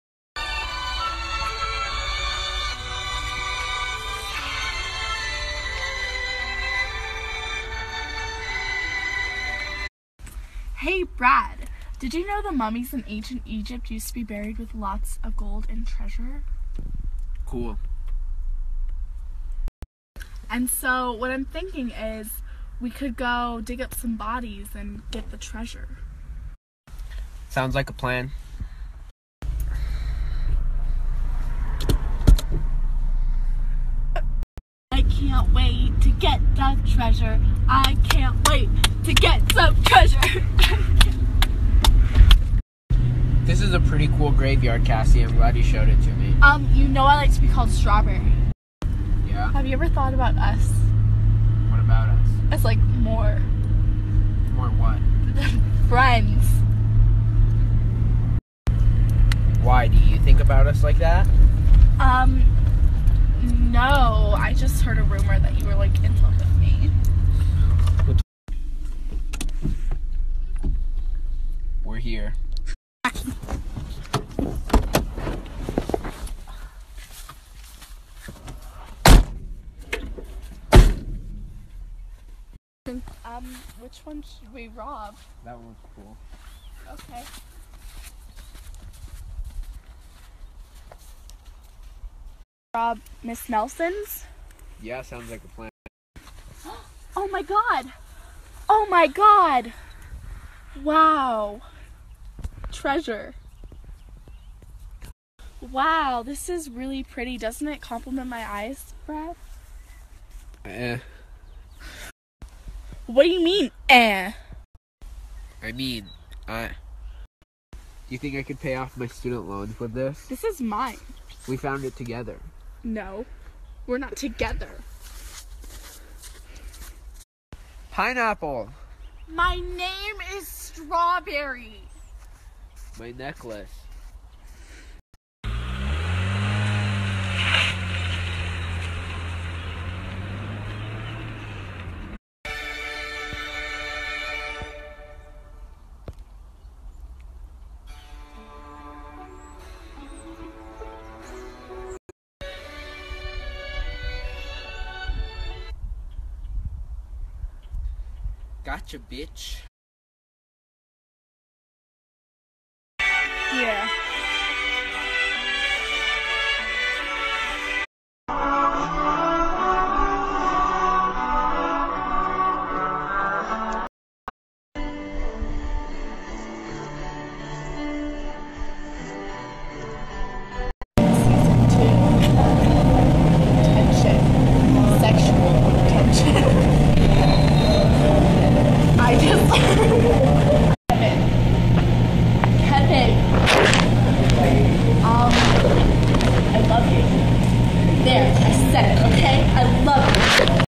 Grave Robbers Movie
Featuring Lexi Killinger as Strawberry and Kevin Cervantes as Brad